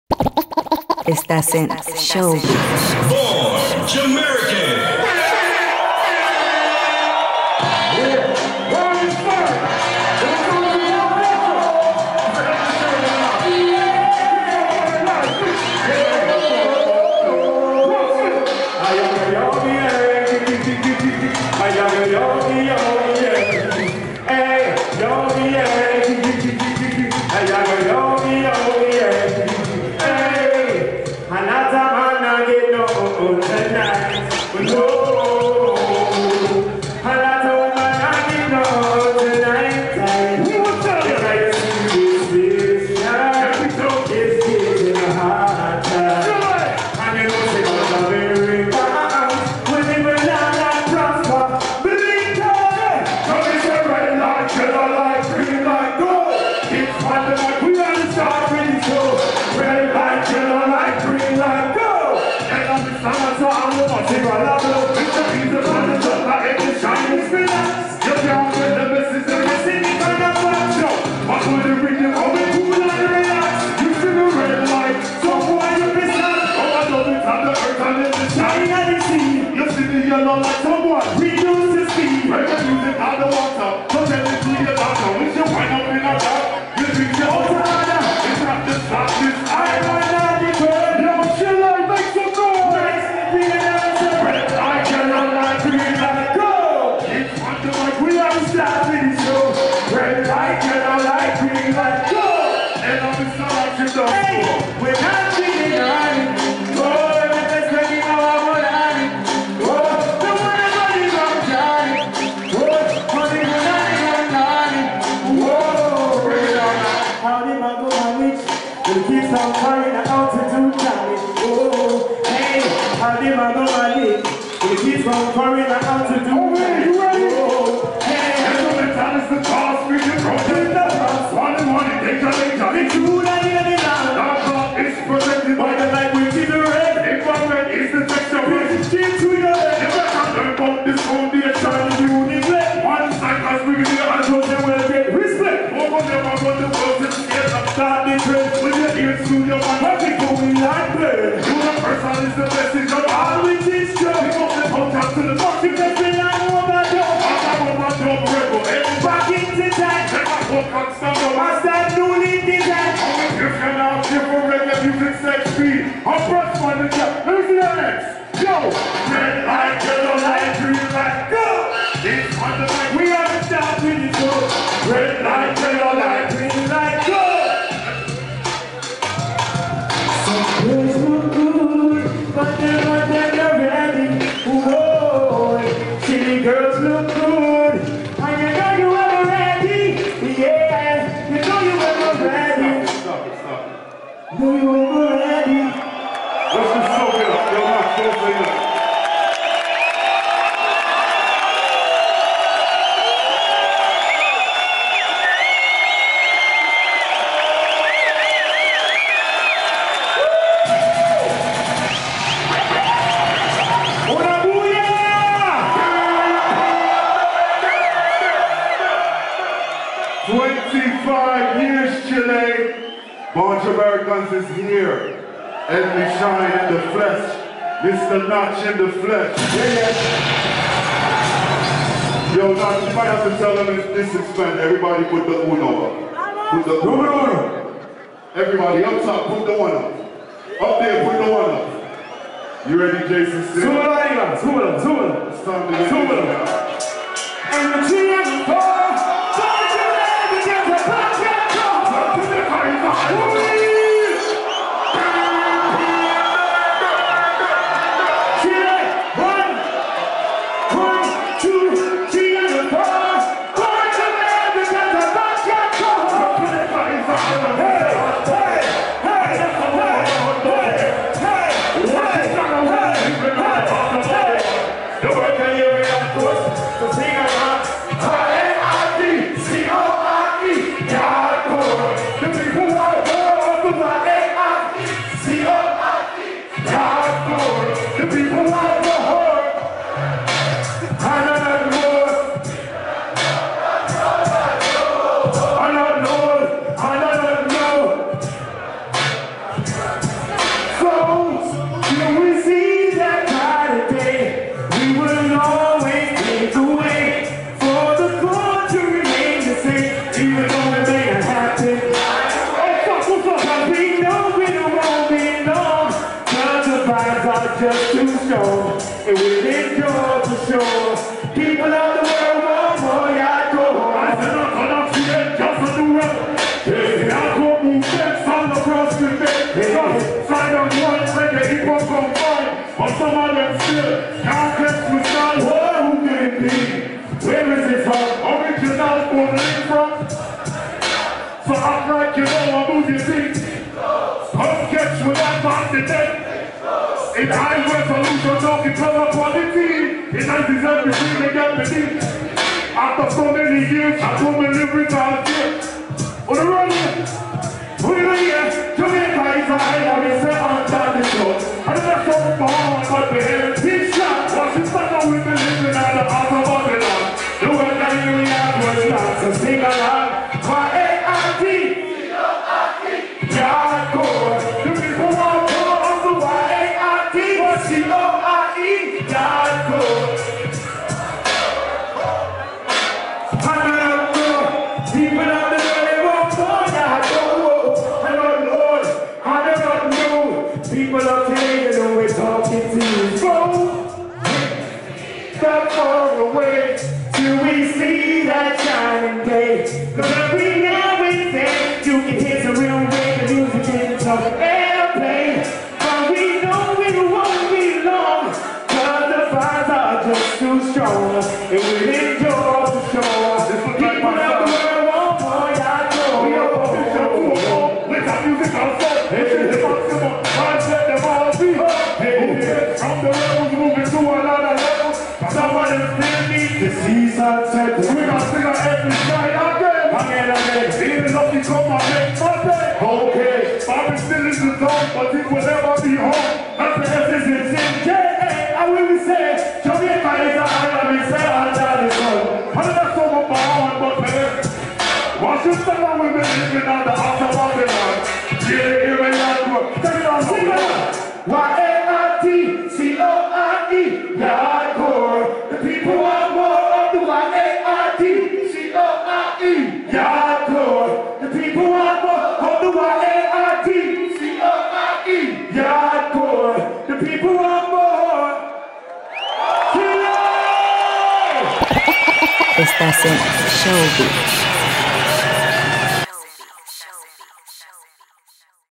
BORN JAMERICANS EN CHILE | SIEMPRE VIVO REGGAE | SHOWBEATS.CL
Te invitamos a ver parte del show de Born Jamericans en Siempre Vivo Reggae 2017.

Producido por Siempre Vivo Reggae.
Prensa: Showbeats.cl